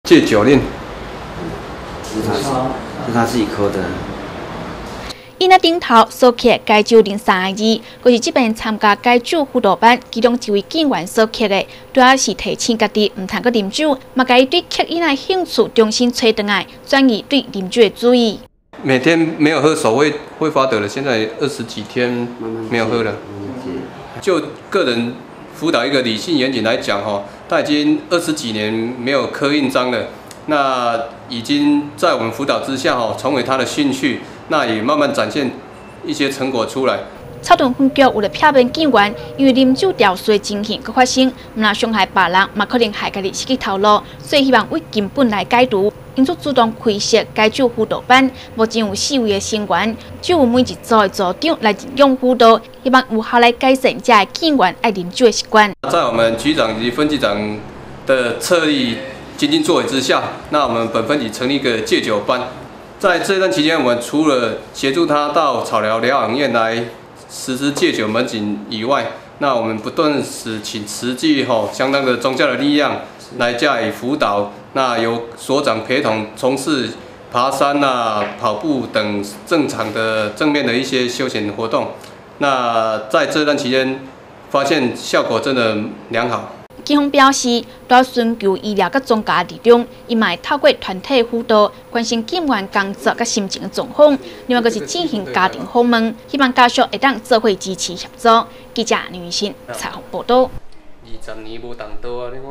南投新聞-草屯分局戒酒輔導班
日前草屯分局一名員警，休假時喝酒肇事，為了避免類似情形再發生，因此分局開設戒酒輔導班，目前有四位成員，透過團體輔導和家庭訪問，以及醫療、宗教的協助，希望幫助這些員警成功戒酒。